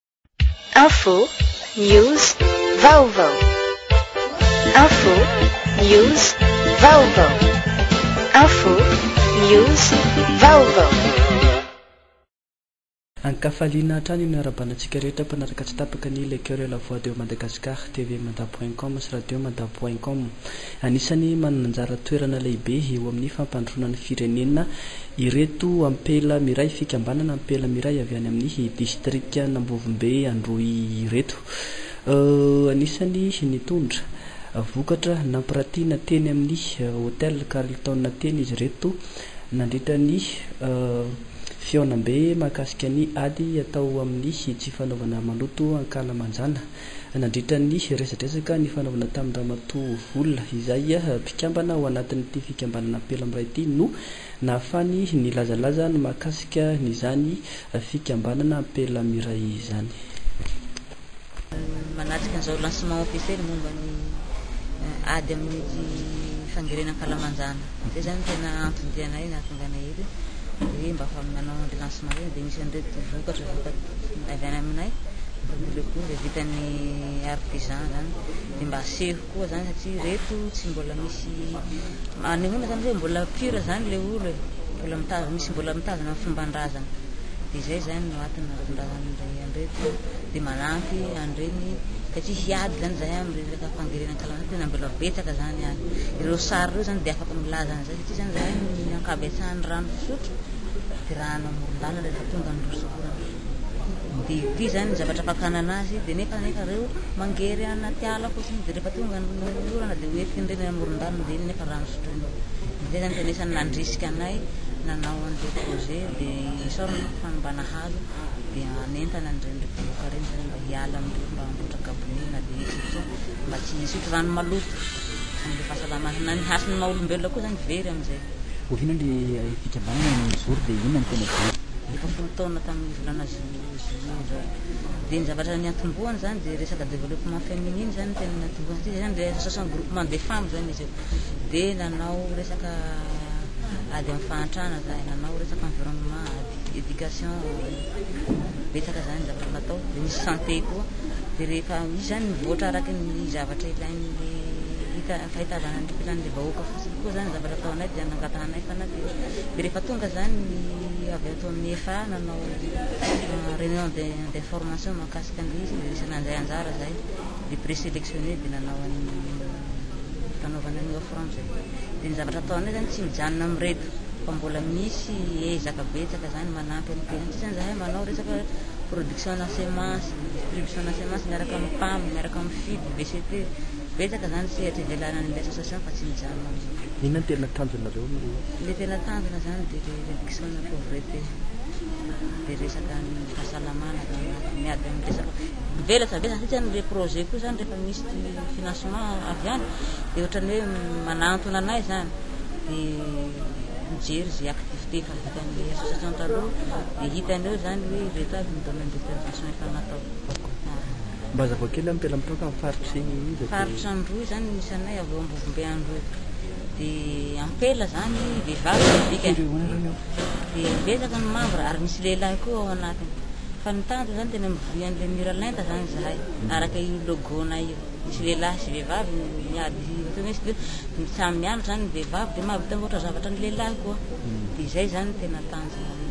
ONG & ASSOCIATIONS : AMPELA MIRAY - LES FEMMES D'ANDROY
Fikambanam-behivavy iray any amin’iny faritra antsimon’ny nosy iny ampela miray, avy ao amin’ny distrikan’i Ambovombe androy. Mandray anjara feno eo amin’ny sehatry ny fampandrosoana ny firenena amin’ny lafiny maro, ara-tsosialy, ara toe-karena.